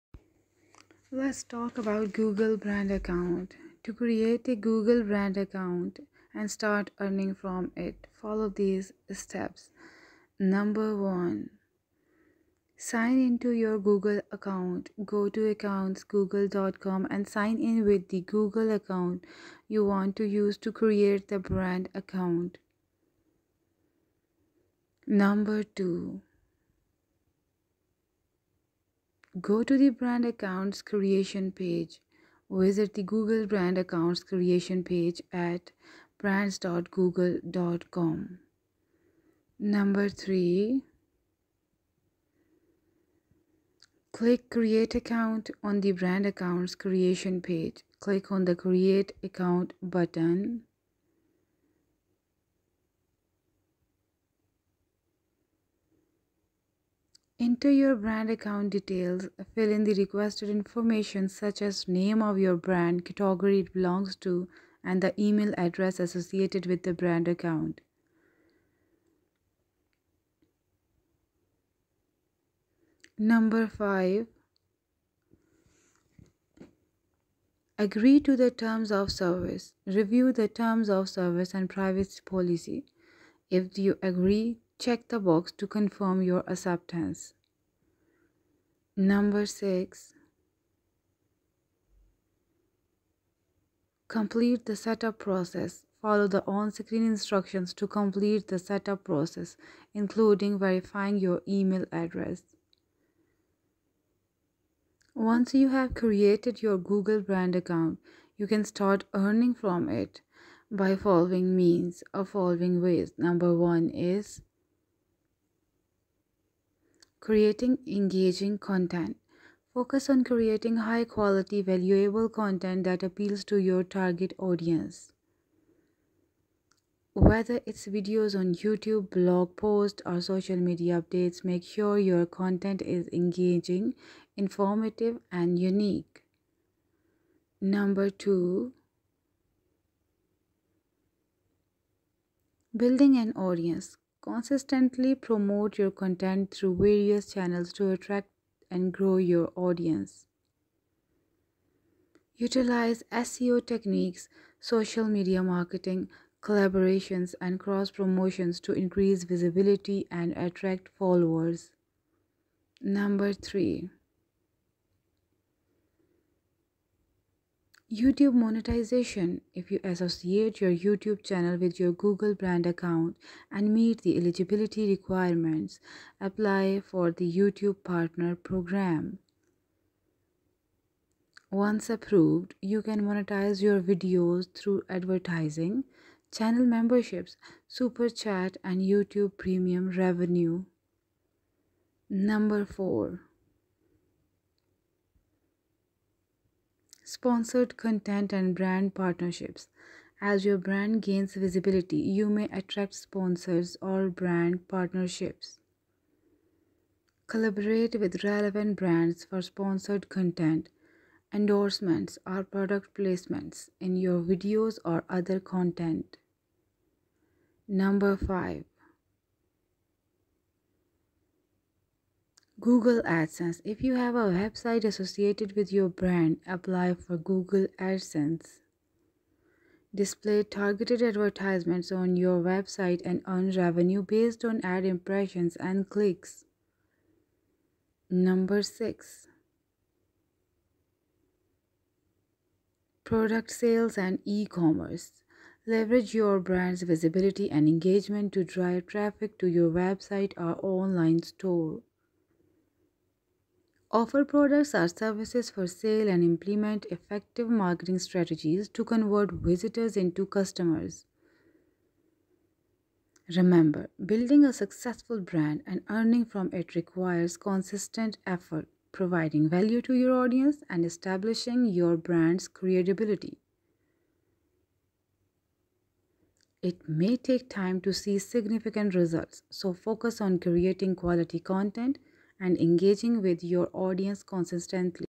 Google Brand Account Guide | Building Your Online Presence 2023
Welcome to [rv124]! In this comprehensive video, we'll explore the concept of the Google Brand Account and guide you step-by-step on how to create one for your own online presence.